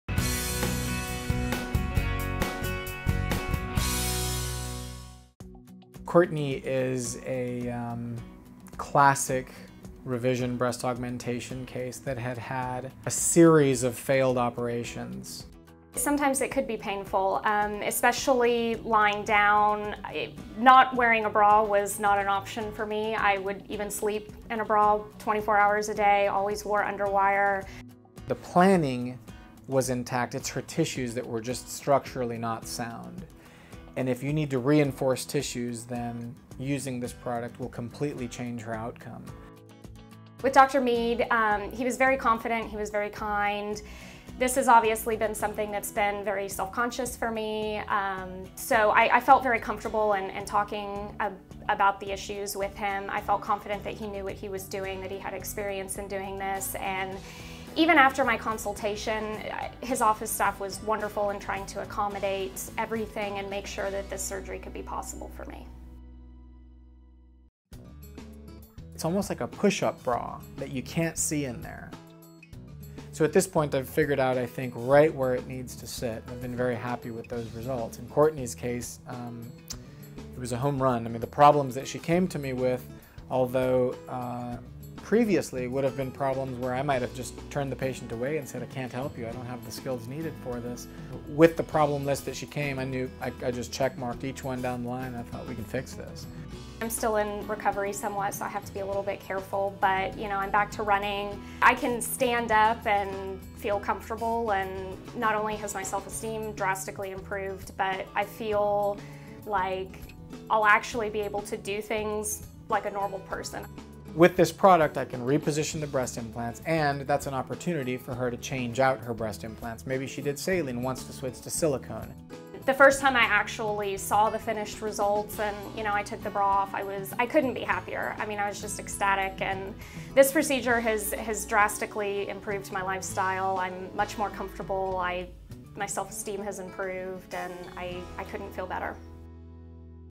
Breast Augmentation Revision with Dallas Plastic Surgeon Dr. Ricardo Meade
Dallas Plastic Surgeon Dr. Ricardo Meade with the Forest Park Medical Center, talks about new technology that allows for better breast augmentation revision surgery. Courtney had undergone multiple failed procedures but now is confident and couldn't be happier.